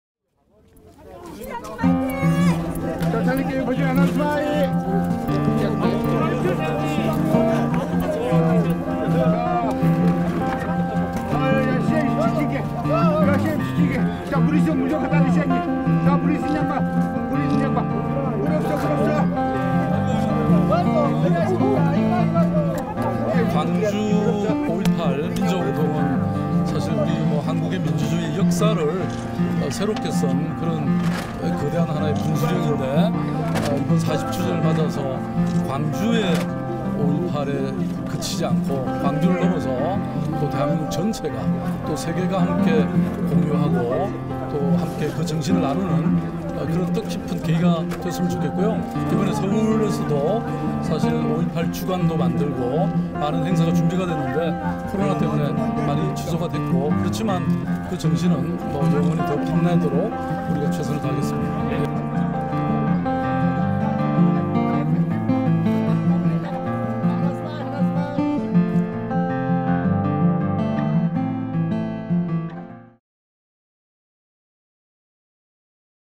고 박원순 시장, 광주와 함께 했던 영상 공개
5.18민주화운동 40주년 기념식에 참석했던 고 박원순 시장은 인권변호사와 시민운동가 출신으로 광주와 많은 인연을 나눴습니다. 고인이 광주시민들과 함께 했던 영상입니다.